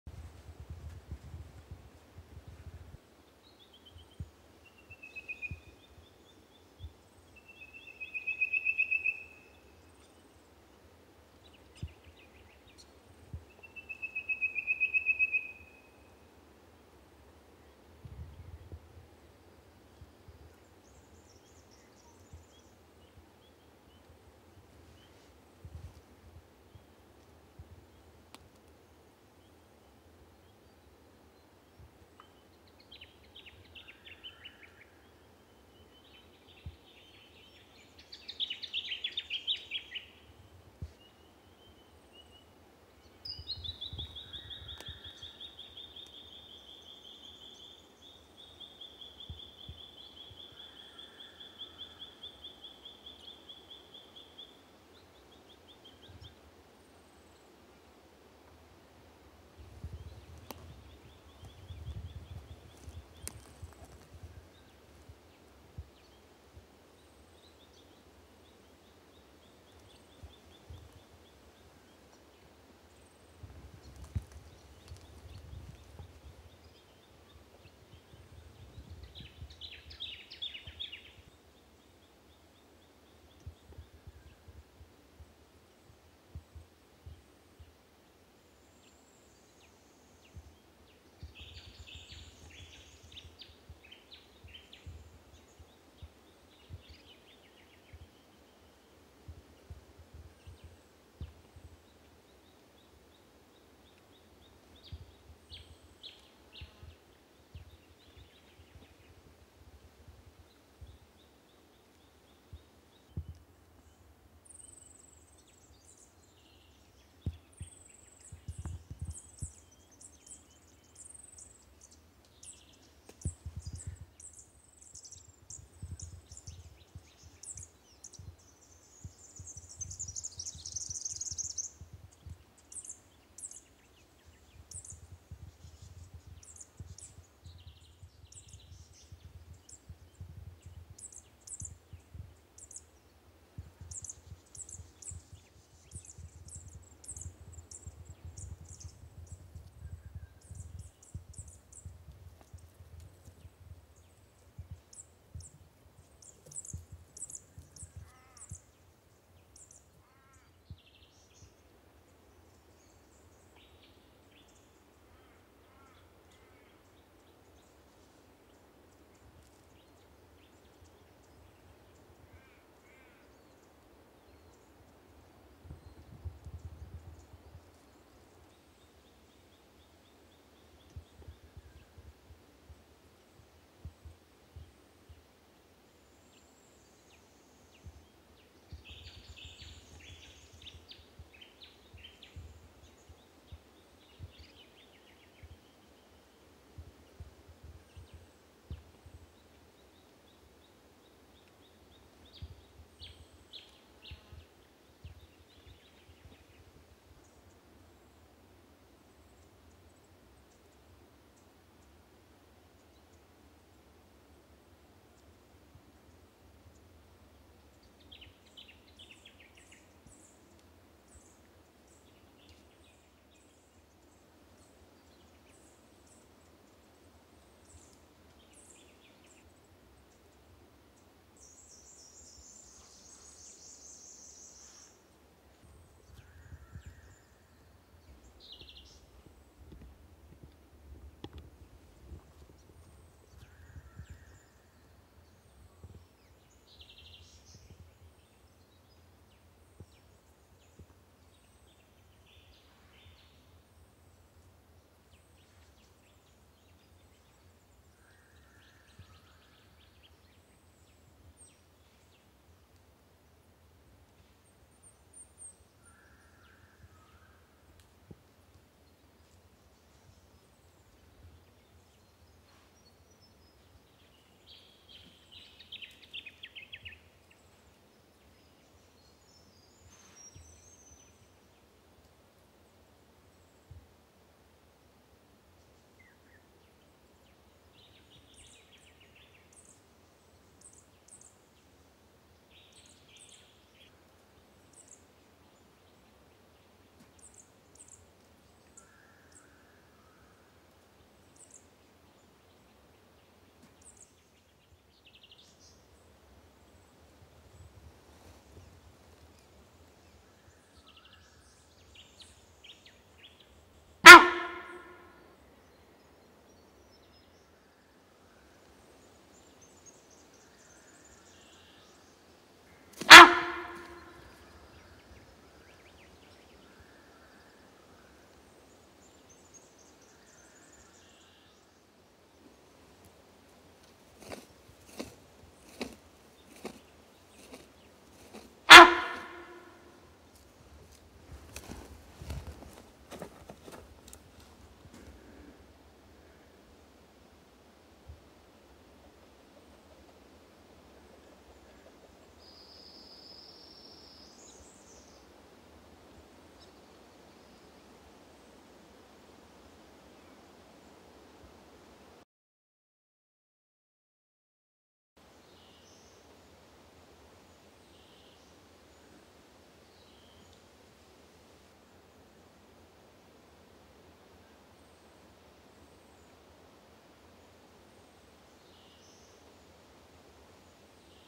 A close encounter with a mature Sambar stag.
An aggressive mature stag being called in by the photographer whilst this mature stag tries to procreate with this hind that's coming into estrus!